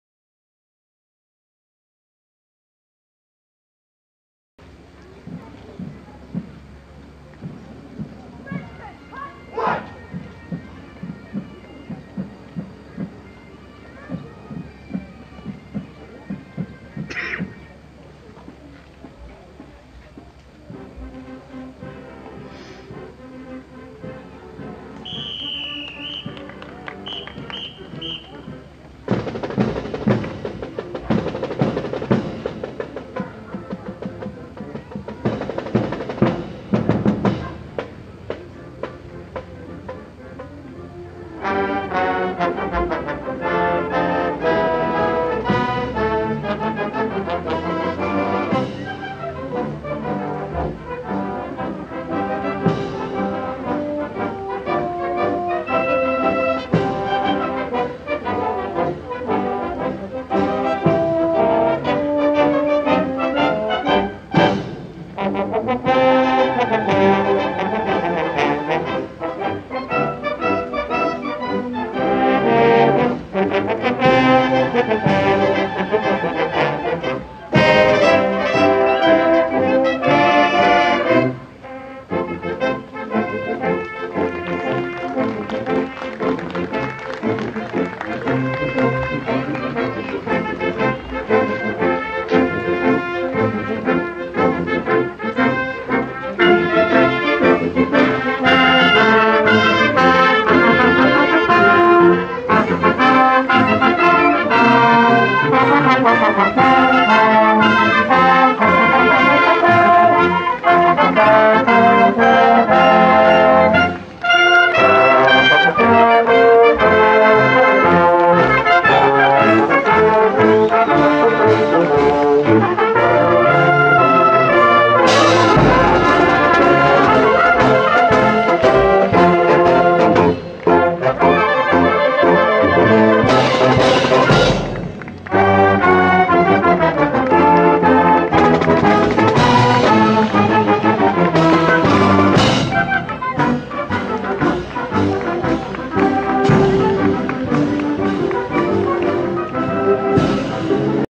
Arlington at Riverside King 10-20-2012
Arlington High School Golden Pride competing at
Riverside King Band Review